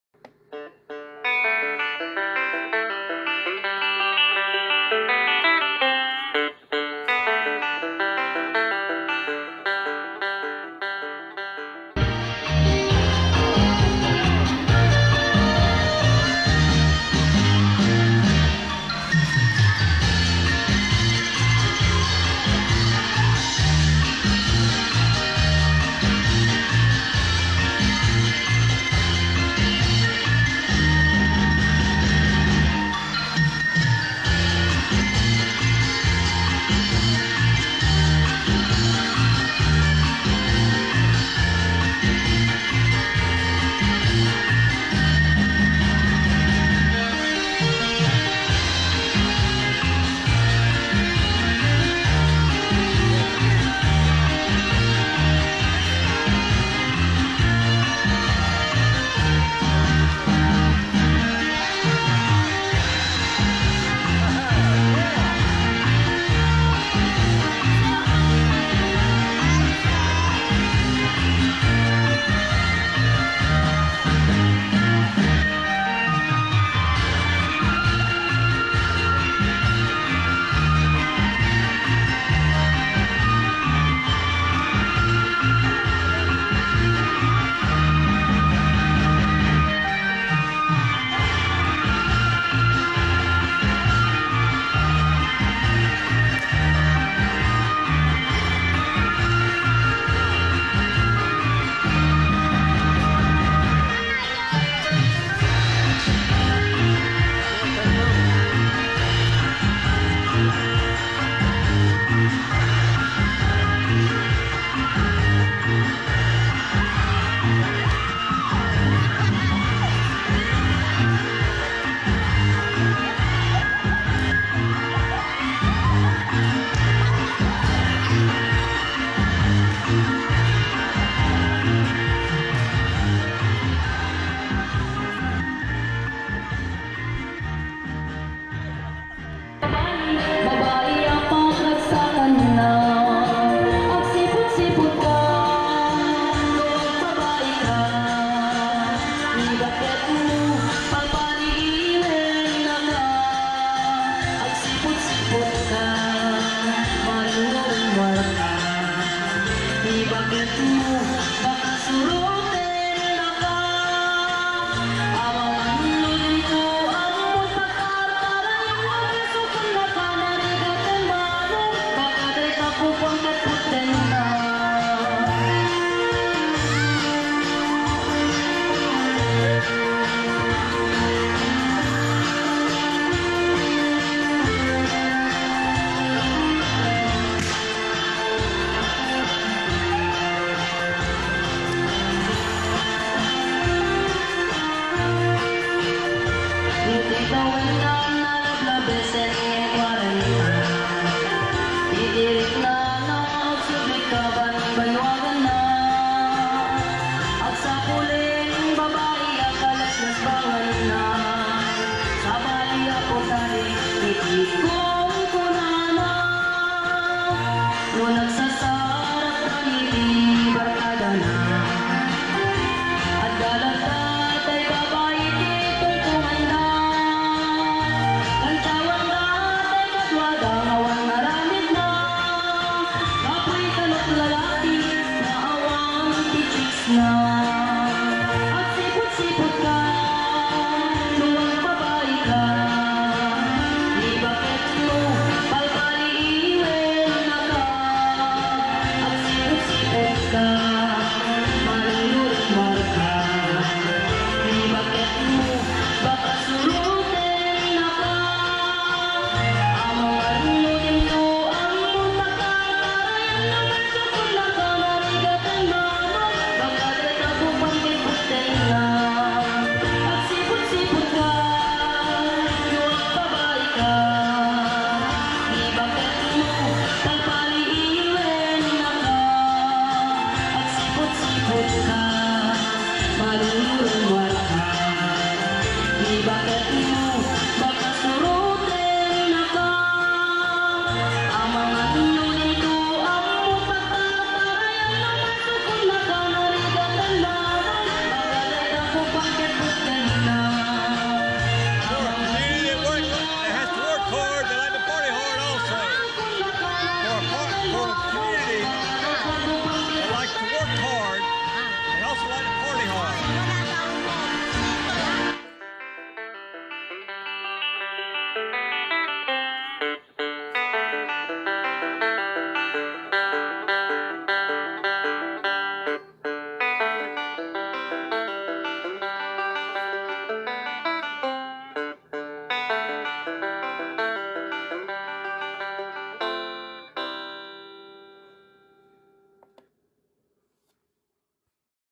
Dance the Night Away! Filipinas Having Fun! – This is Our Philippines
Dance, dance and dance some more! In our Barangay the ladies work really hard. Often from sunup to sundown they are working. Seldom do they get to let their hair down and have some fun! Christmas is one of those times.

Sweet home music on the banjo!